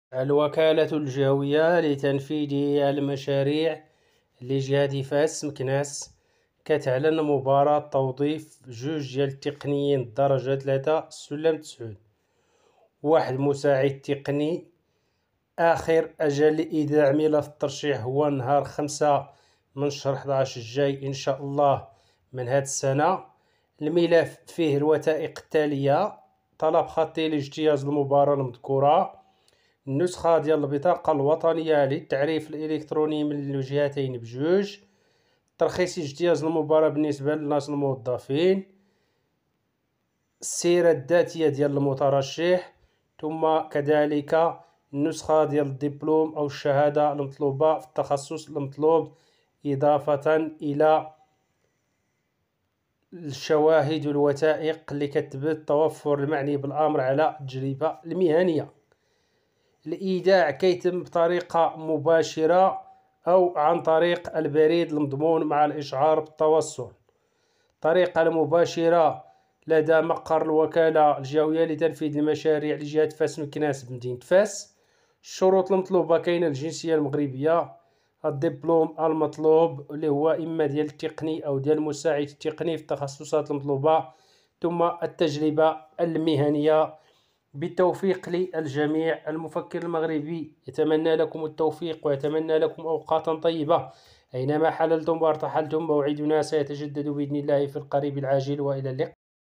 التوظيف في المؤسسات العمومية:الوكالة الجهوية لتنفيذ المشاريع لجهة فاس مكناس تنظم مباراة توظيف
التوظيف في المؤسسات العمومية: الوكالة الجهوية لتنفيذ المشاريع لجهة فاس مكناس تنظم مباراة توظيف#المفكر_المغربي_منكم_وإليكم_ومعكم#الوظيفة#العمل#الخدمة#الأجرة#الصالير#الباكالوريا#الاجازة#الماستر#الدكتوراة#الدستور#الهجرة#اكسبلور#الجميع#الشروط#المسطرة